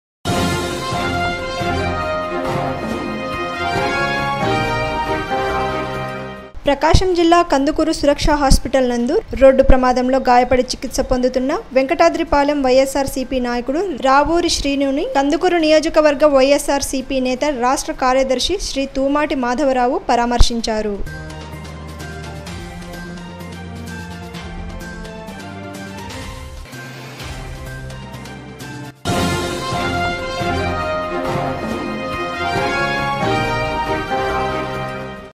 కందుకూరు రావూరి శ్రీనుని పరామర్శించిన కందుకూరు నియోజకవర్గ YSRCP నేత రాష్ట్ర కార్యదర్శి శ్రీ తూమాటి
ప్రకాశం జిల్లా కందుకూరు న్యూస్ :- కందుకూరు సురక్ష హాస్పిటల్ నందు రోడ్డు ప్రమాదంలో గాయపడి చికిత్స పొందుతున్న వెంకటాద్రిపాలెం వైఎస్సార్సీపీ
 నాయకుడు రావూరి.శ్రీను గారిని   పరామర్శించిన కందుకూరు నియోజకవర్గ వైఎస్ఆర్సీపీ నేత రాష్ట్ర కార్యదర్శి శ్రీ తూమాటి. మాధవరావు గారు తదితరులు పాల్గొన్నారు...!